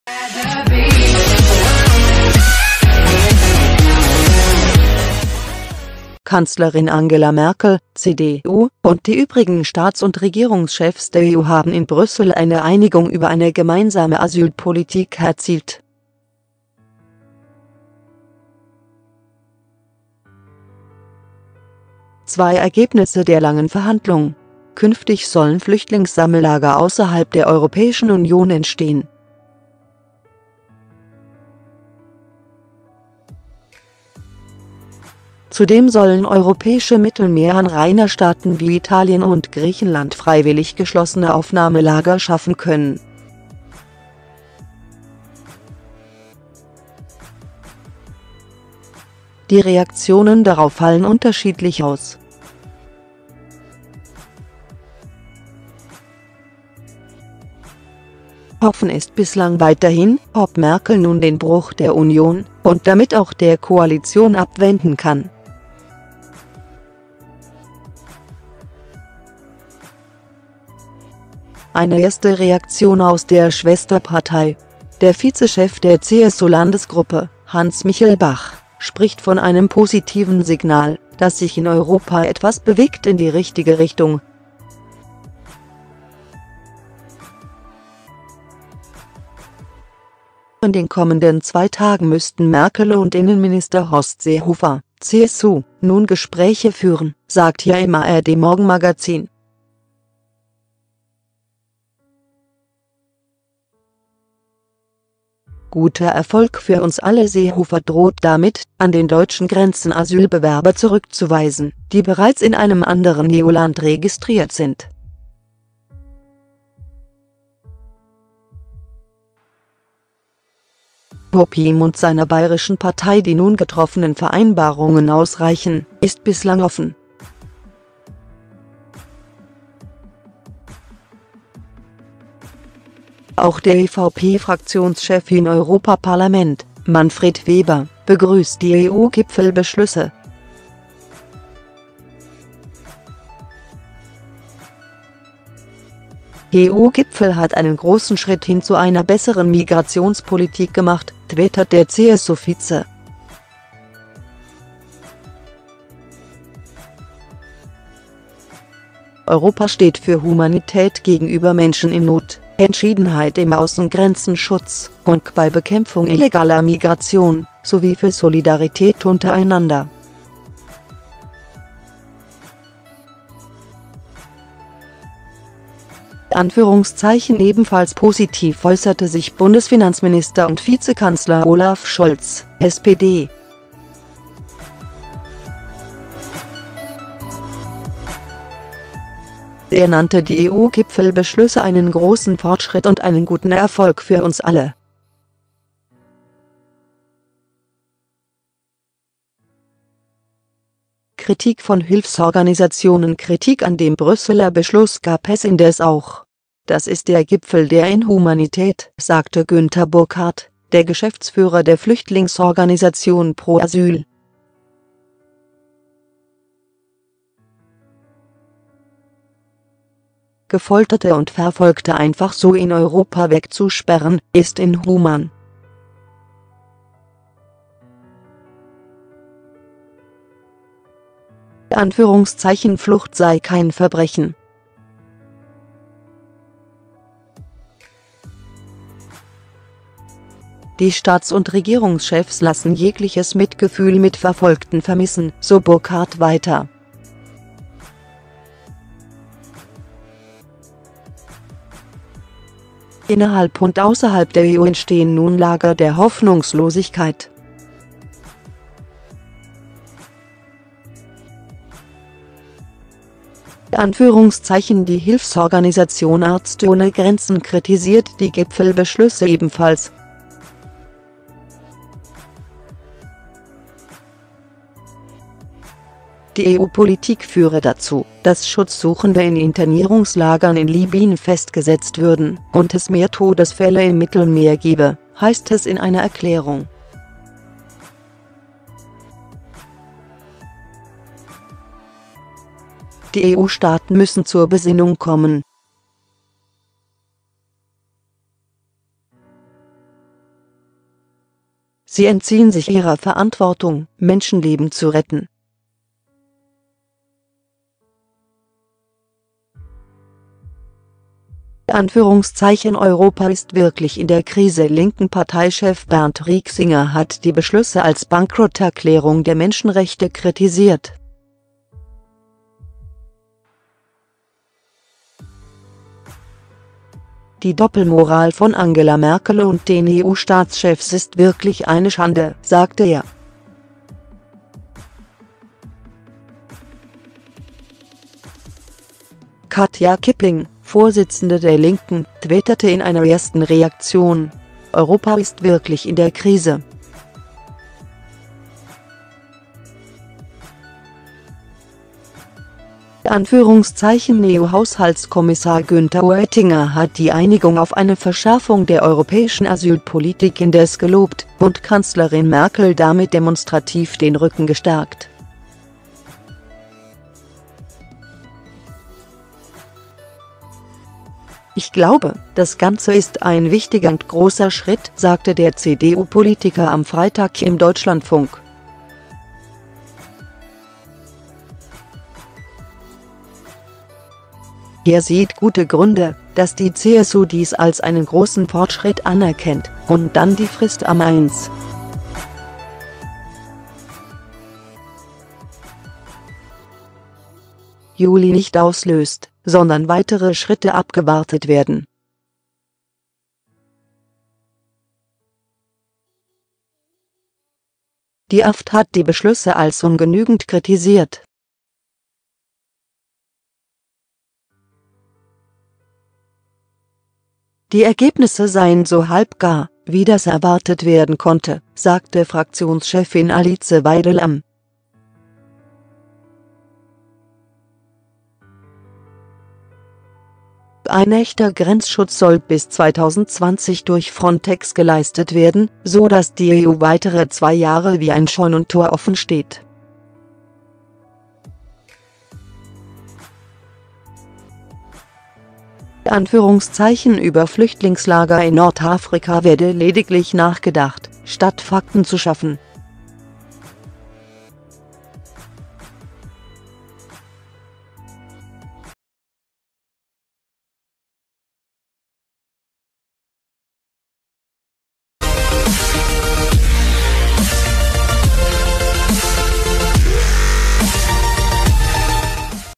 [Schock] | Reaktionen auf EU-Asyleinigung: "Gipfel der Inhumanität" oder "großer Schritt" - SPIEGEL
[Tags]
 Kanzlerin Angela Merkel (CDU) und die übrigen Staats- und Regierungschefs der EU haben in Brüssel eine Einigung über eine gemeinsame Asylpolitik erzielt. Zwei Ergebnisse der langen Verhandlung:   Künftig sollen Flüchtlings-Sammellager außerhalb der Europäischen Union entstehen.   Zudem sollen europäische Mittelmeeranrainerstaaten wie Italien und Griechenland freiwillig geschlossene Aufnahmelager schaffen können. Die Reaktionen darauf fallen unterschiedlich aus.   Offen ist bislang weiterhin, ob Merkel nun den Bruch der Union und damit auch der Koalition abwenden kann. Eine erste Reaktion aus der Schwesterpartei: Der Vizechef der CSU-Landesgruppe, Hans Michelbach, spricht von einem "positiven Signal, dass sich in Europa etwas bewegt in die richtige Richtung". In den kommenden zwei Tagen müssten Merkel und Innenminister Horst Seehofer (CSU) nun Gespräche führen, sagt er im ARD-Morgenmagazin.     "Guter Erfolg für uns alle"    Seehofer droht damit, an den deutschen Grenzen Asylbewerber zurückzuweisen, die bereits in einem anderen EU-Land registriert sind. Ob ihm und seiner bayerischen Partei die nun getroffenen Vereinbarungen ausreichen, ist bislang offen.   Auch der EVP-Fraktionschef im Europaparlament, Manfred Weber, begrüßt die EU-Gipfelbeschlüsse. "EU-Gipfel hat einen großen Schritt hin zu einer besseren Migrationspolitik gemacht", twittert der CSU-Vize. "Europa steht für Humanität gegenüber Menschen in Not, Entschiedenheit im Außengrenzenschutz und bei Bekämpfung illegaler Migration sowie für Solidarität untereinander."    Ebenfalls positiv äußerte sich Bundesfinanzminister und Vizekanzler Olaf Scholz (SPD). Er nannte die EU-Gipfelbeschlüsse einen "großen Fortschritt und einen guten Erfolg für uns alle".    Kritik von Hilfsorganisationen    Kritik an dem Brüsseler Beschluss gab es indes auch: "Das ist der Gipfel der Inhumanität", sagte Günter Burkhardt, der Geschäftsführer der Flüchtlingsorganisation Pro Asyl. "Gefolterte und Verfolgte einfach so in Europa wegzusperren, ist inhuman." Flucht sei kein Verbrechen.   "Die Staats- und Regierungschefs lassen jegliches Mitgefühl mit Verfolgten vermissen", so Burkhardt weiter. "Innerhalb und außerhalb der EU entstehen nun Lager der Hoffnungslosigkeit."   Die Hilfsorganisation Ärzte ohne Grenzen kritisiert die Gipfelbeschlüsse ebenfalls. Die EU-Politik führe dazu, dass Schutzsuchende in Internierungslagern in Libyen festgesetzt würden und es mehr Todesfälle im Mittelmeer gebe, heißt es in einer Erklärung. "Die EU-Staaten müssen zur Besinnung kommen. Sie entziehen sich ihrer Verantwortung, Menschenleben zu retten."    "Europa ist wirklich in der Krise"    Linken-Parteichef Bernd Riexinger hat die Beschlüsse als "Bankrotterklärung der Menschenrechte" kritisiert. "Die Doppelmoral von Angela Merkel und den EU-Staatschefs ist wirklich eine Schande", sagte er. Katja Kipping, Vorsitzende der Linken, twitterte in einer ersten Reaktion: "Europa ist wirklich in der Krise."   EU-Haushaltskommissar Günther Oetting